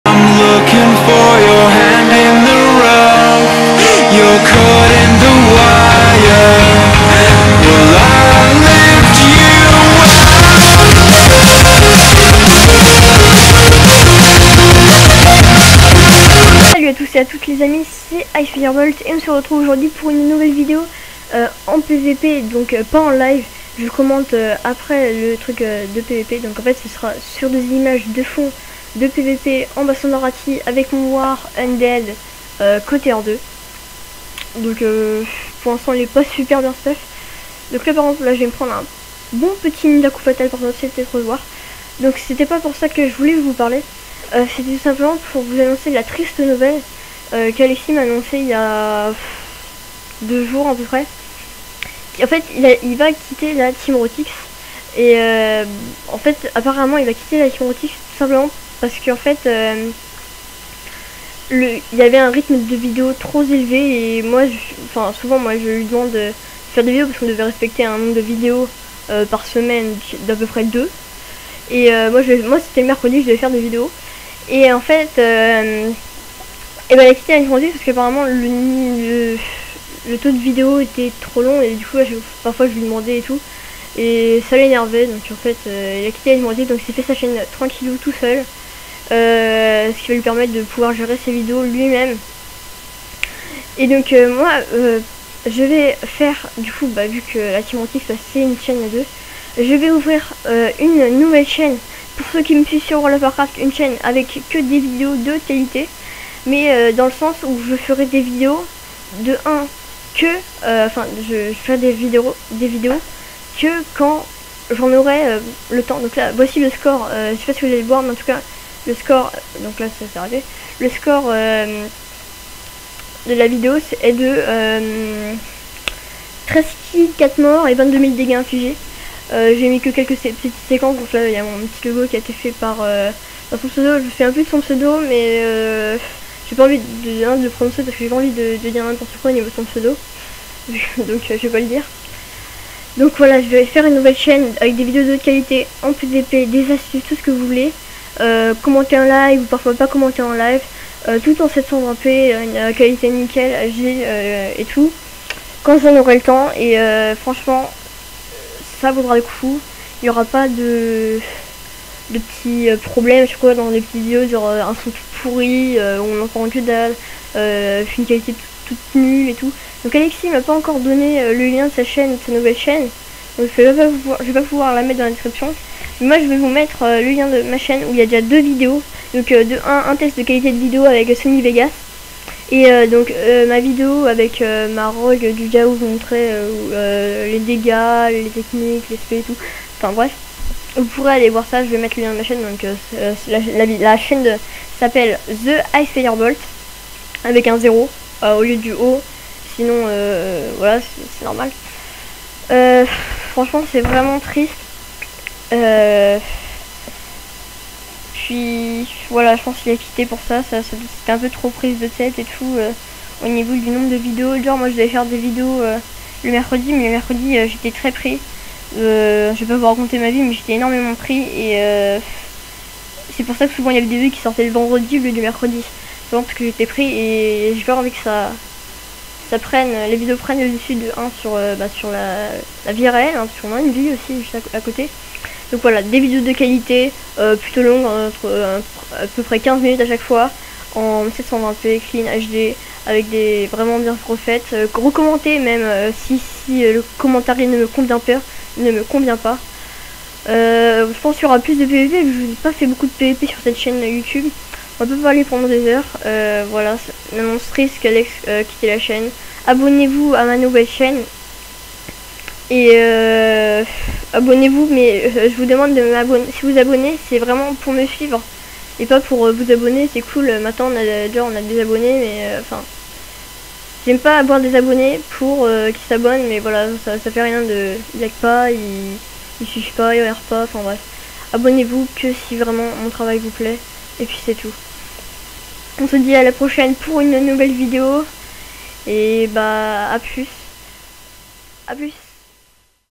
Un grand changement et une annonce triste :(
Yo tt le monde aujourd'hui une vidéo qui sera surement une des dernière SUR CETTE CHAINE car Alex7373100 de la teamrotyx quittent la teamrotyx.
Mais je ne vous demande pas de vous abonnez simple et bètement je veux surtout des gens qui me suivent sinon l'abonnement ne sert a rien et quand je parle de suivre c'est juste le petit like et le com sur ce que vous en pensez !
a+ et j'èspere que vous me suivrez sur ma nouvelle chaine :) .